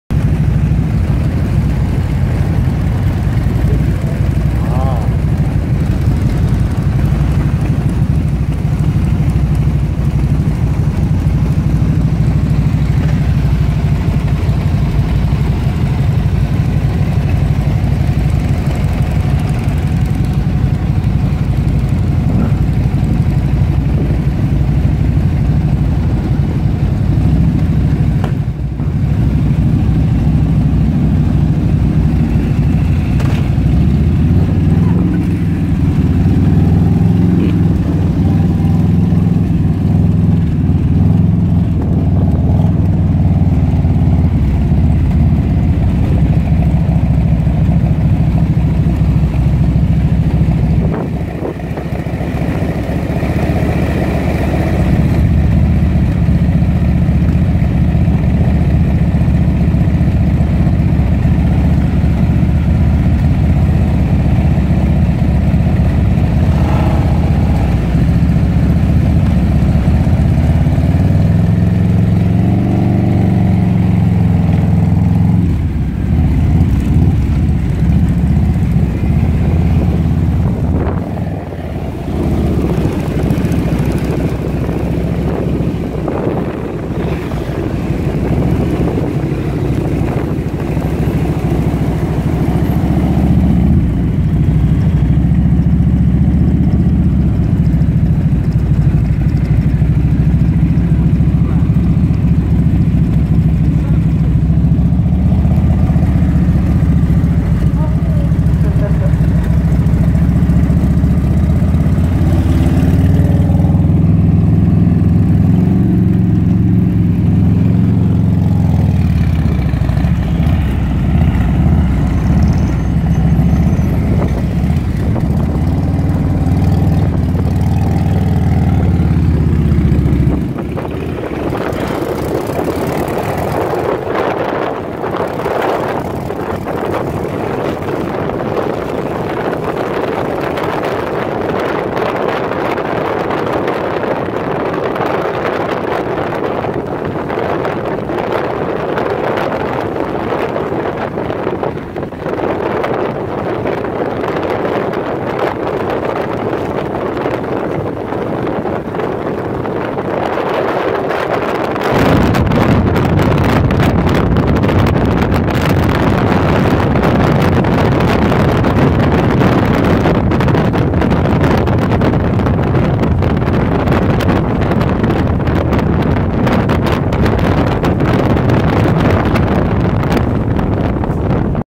A Ride Out for Rob's 50th birthday
A rideout to newlandscorner in surrey england for Rob's 50thbirthday in #2018
I was pillion on Rob's harleydavidson electraglide flhs, there was a variety of motorbikes, including a buellcyclone